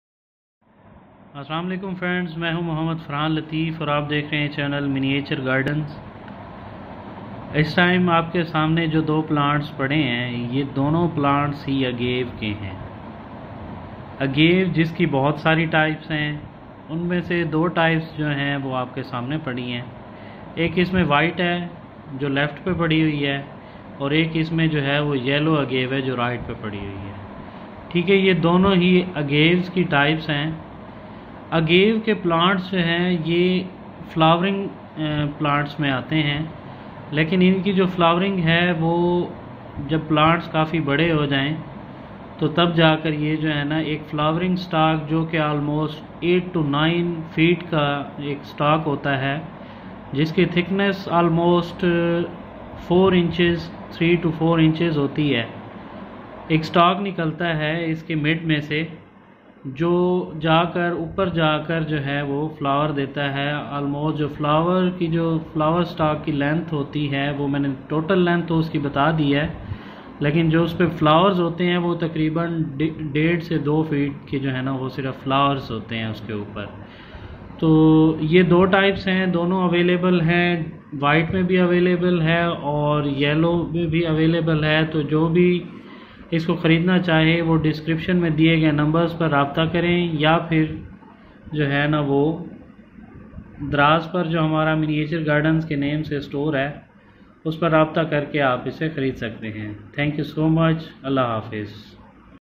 Agave plants. Live plants. White and yellow agaves.
1. Two varieties of agaves.
2. White and yellow.
3. Flowering plants.
4. If anyone want to purchase please contact on these numbers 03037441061 / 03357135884.
5. Miniature Gardens (Plants and More).